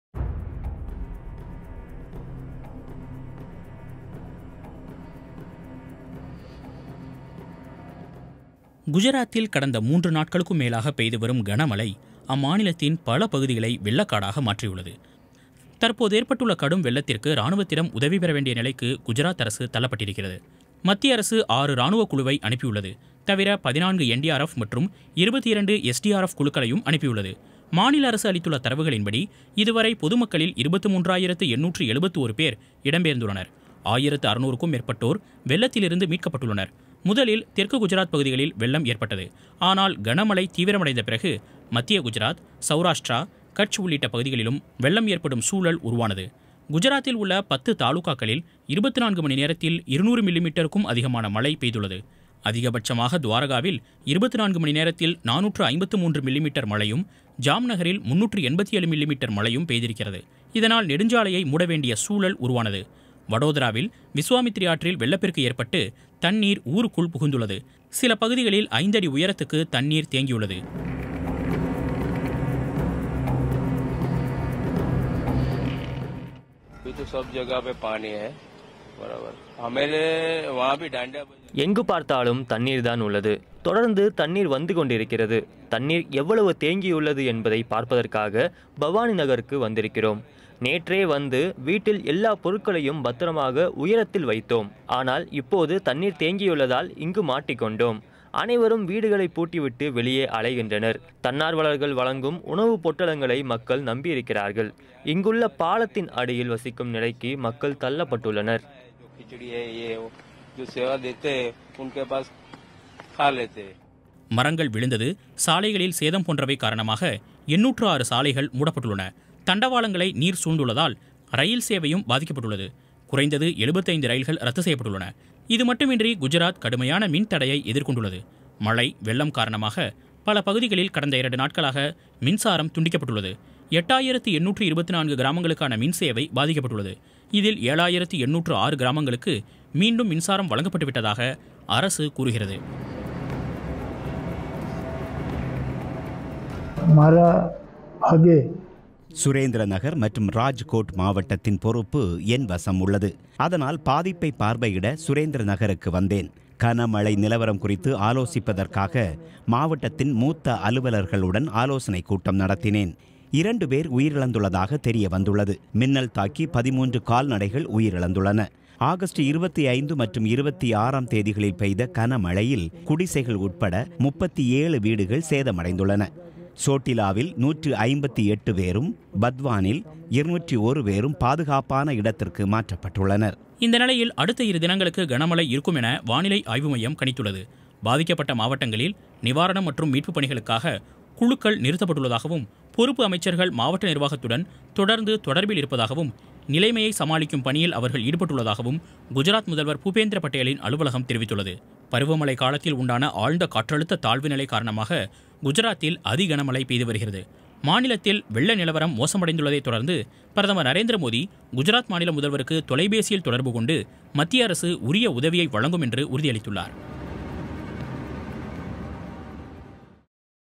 வெள்ளக்காடான Gujarat; CM-க்கு Phone போட்ட PM; விரைந்த Army, NDRF - என்ன நடக்கிறது?
குஜராத்தில் கடந்த 3 நாட்களுக்கும் மேலாக பெய்துவரும் கனமழை அம்மாநிலத்தின் பல பகுதிகளை வெள்ளக்காடாக மாற்றியுள்ளது. 10 மாவட்டங்கள் வெள்ளத்தின் பிடியில் சிக்கியுள்ளன.  தற்போது ஏற்பட்டுள்ள கடும் வெள்ளத்திற்கு ராணுவத்திடம் உதவிபெற வேண்டிய நிலைக்கு தள்ளப்பட்டுள்ளது. மத்திய அரசு ஆறு ராணுவ குழுவை அனுப்பியுள்ளது. தவிர, 14 தேசிய பேரிடர் மீட்புப் படை  மற்றும் 22 மாநில பேரிடர் மீட்புப்படை குழுக்களை அனுப்பியுள்ளது.

 மாநில அரசு அளித்துள்ள தரவுகளின்படி, இதுவரை பொதுமக்களில் 23, 871 பேர் இடம் பெயர்ந்துள்ளனர். 1600க்கும் மேற்பட்டோர் வெள்ளத்தில் இருந்து மீட்கப்பட்டுள்ளனர். முதலில் தெற்கு குஜராத் பகுதிகளில் வெள்ளம் ஏற்பட்டது, ஆனால் கன மழை தீவிரமடைந்த பிறகு மத்திய குஜராத், சௌராஷ்டிரா, கட்ச் உள்ளிட்ட பகுதிகளிலும் வெள்ளம் ஏற்படும் சூழல் உருவானது.

இது, பிபிசிக்காக கலெக்டிவ் நியூஸ்ரூம் வெளியீடு